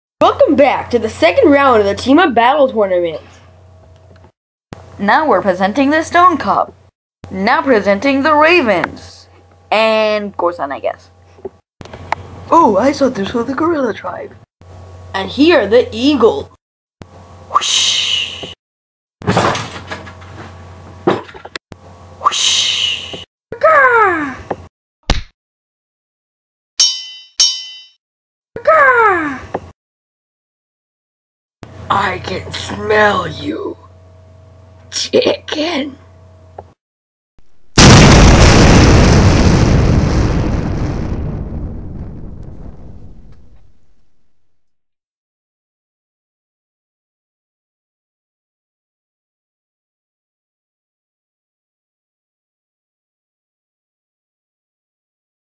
Lego Chima tournaments #2: Ravens vs Eagles
Were back in a less choppy video, now the eagles and ravens are fighting it out. I hope you like it!